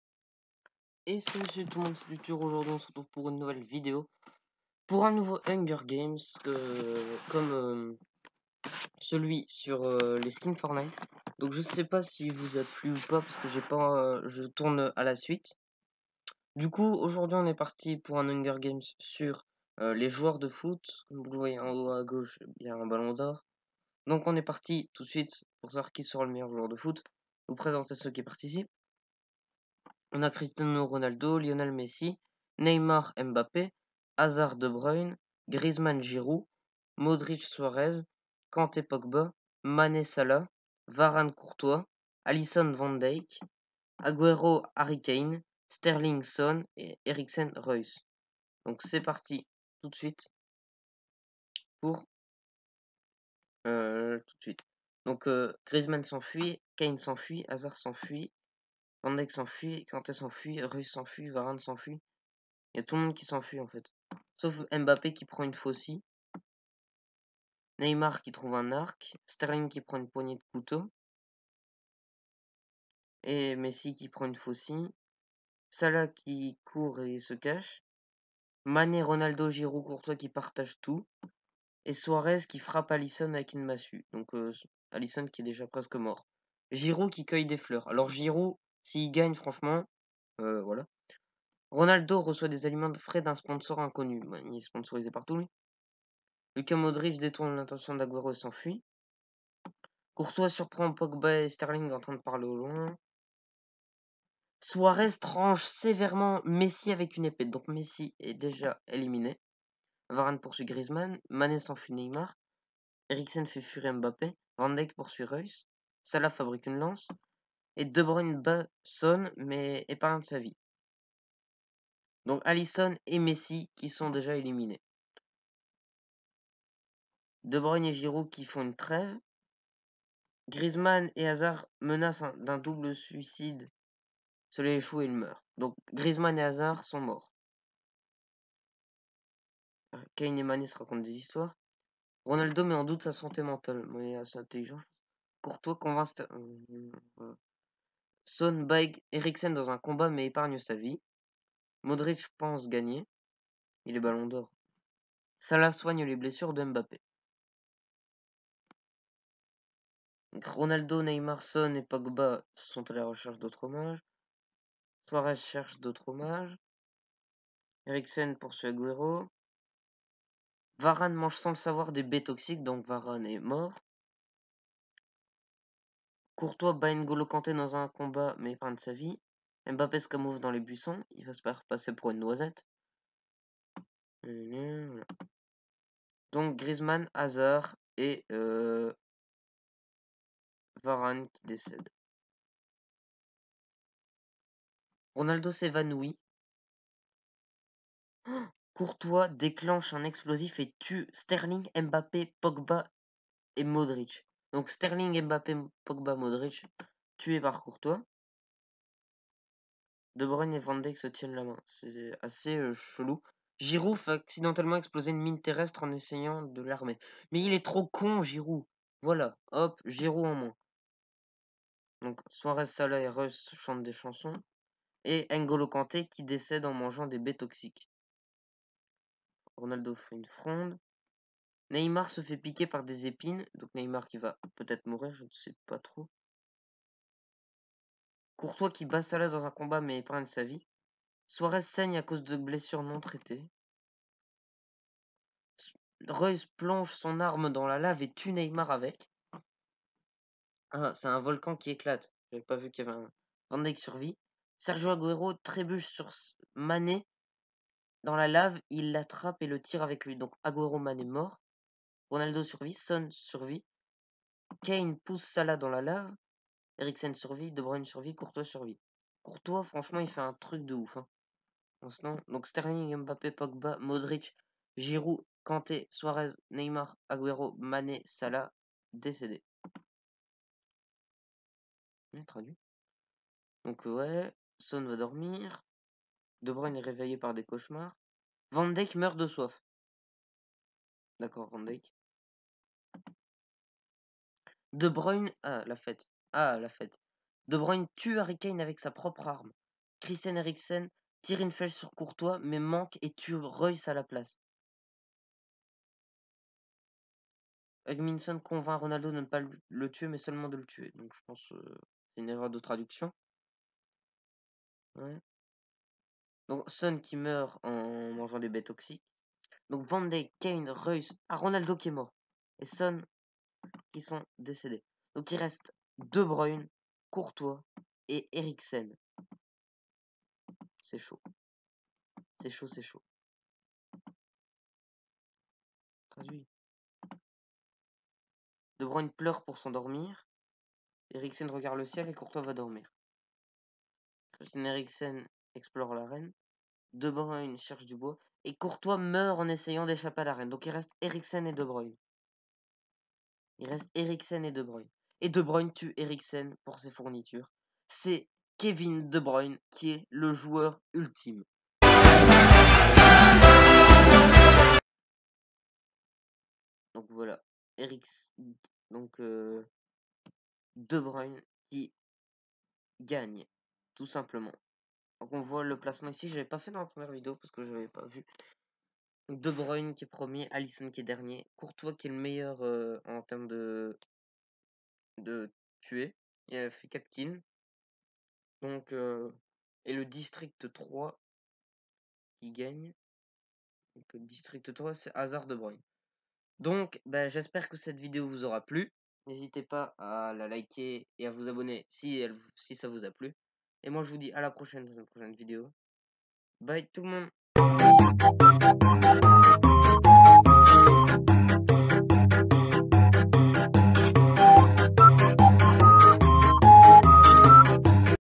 QUI SERA LE MEILLEUR JOUEUR DE FOOT ? - HUNGER GAMES FOOTBALL
Aujourd'hui, les meilleurs joueurs de foot se retrouvent sur une île mais un seul gagnera bref c'est un hunger games mais entre les meilleurs joueurs de foot pour savoir qui sera le joueur ultime